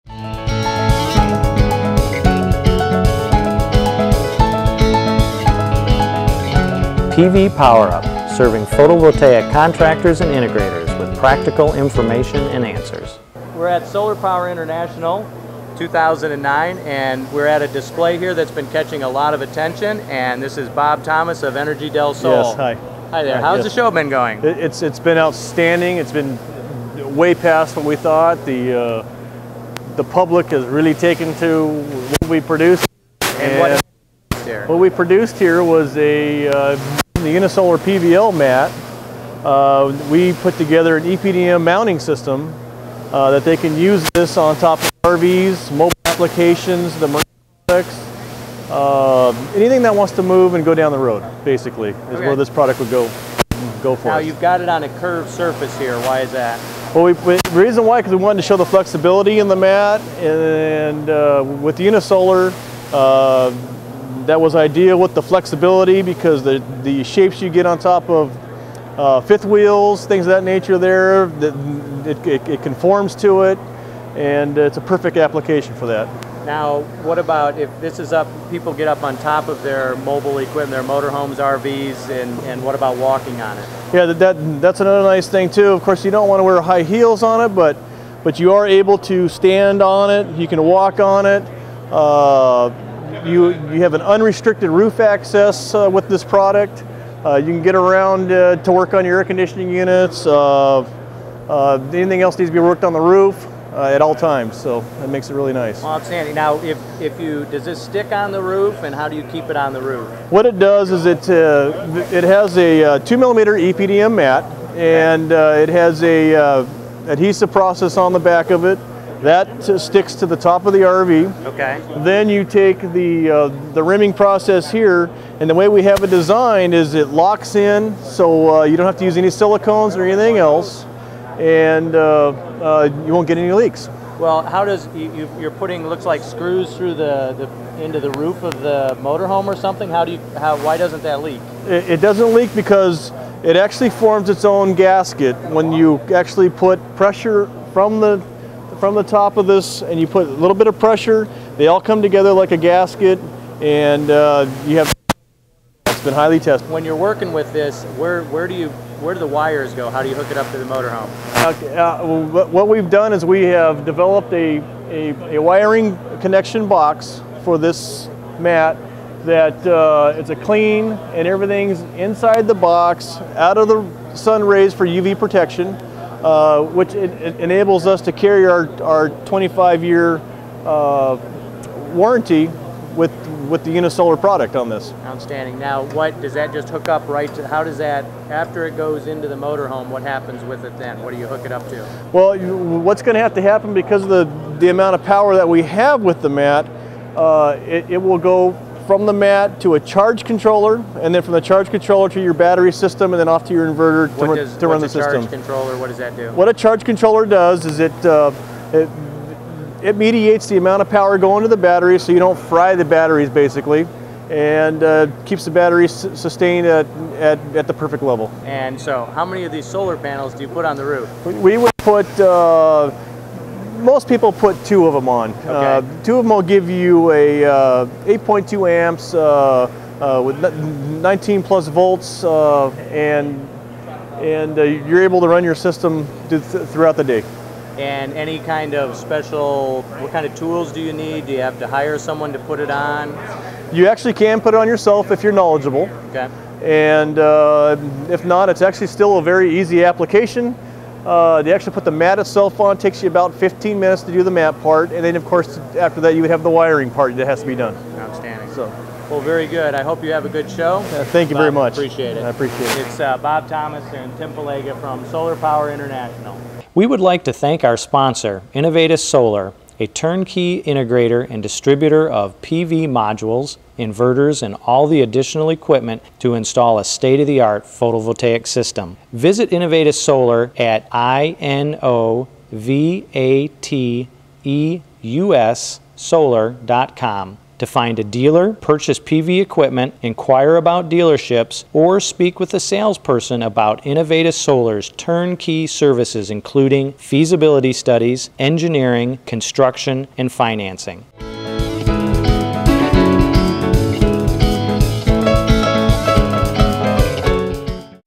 PVpowerUP Episode 10 - Interview With Energy Del Sol
Tim is once again at Solar Power International. This time he talks to Bob Thomas of Energy Del Sol. Energy Del Sol specializes in attaching solar panels to RVs. Bob explains a bit about the process of attaching the panels.